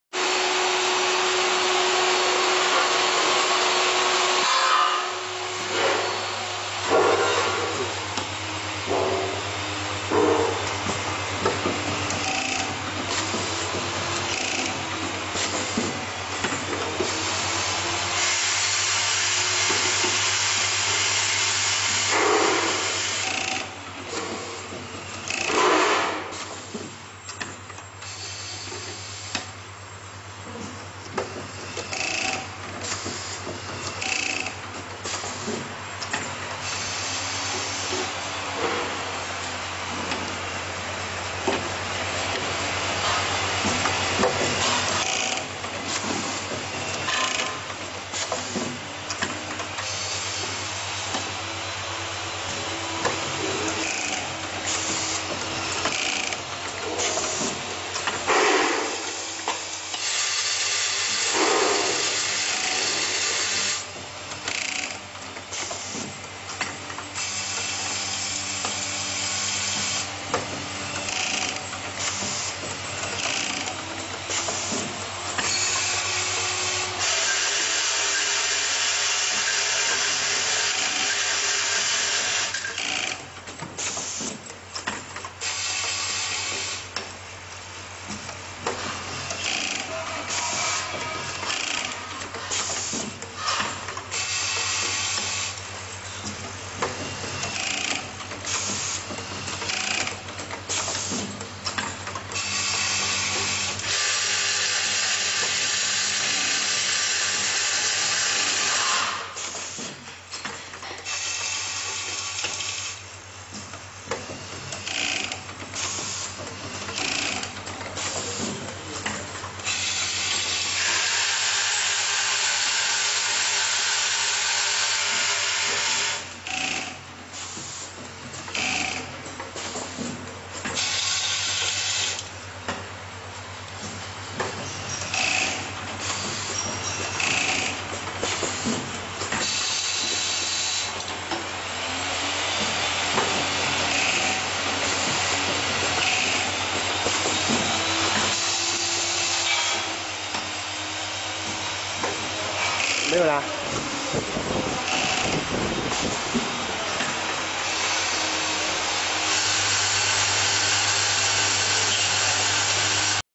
Automatic assembly and screw fitting machie
This machine is special for assembly and fitting screw for motor of sewing machine.